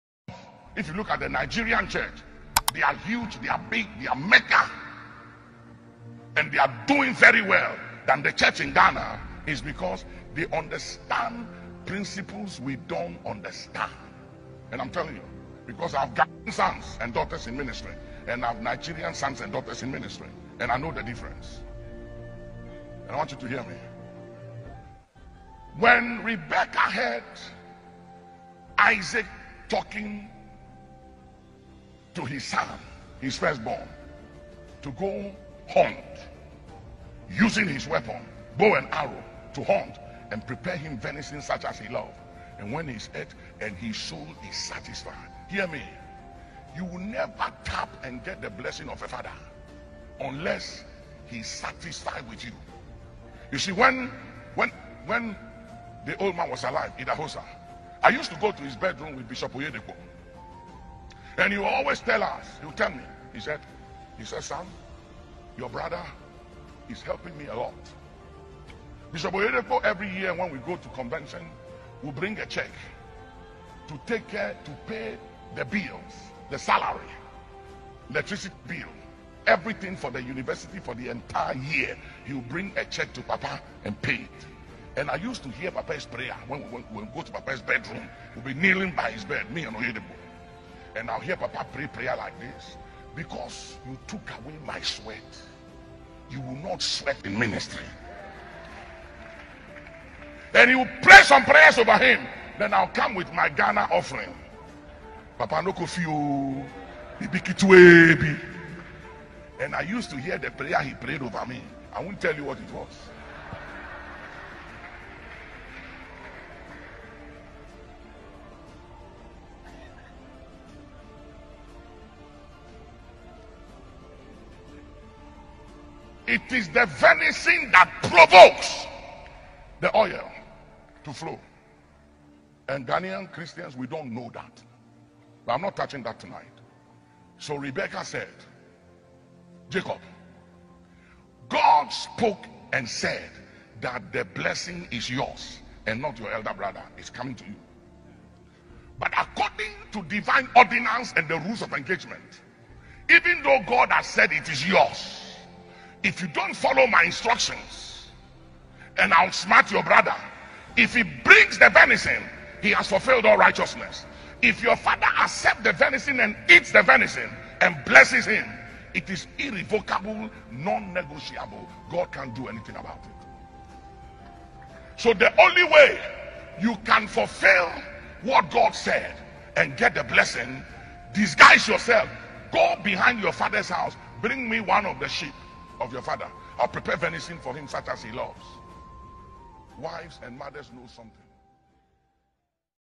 THIS IS WHAT ARCHBISHOP DUNCAN-WILLIAMS SAID ABOUT BISHOP OYEDEPO AND THEIR SPIRITUAL FATHER
Sunrise Tv brings to you again another exclusive clip from the Action chapel International, Spintex Road. In this clip, his Eminence Archbishop Duncan-Williams explain more on the blessing of a father.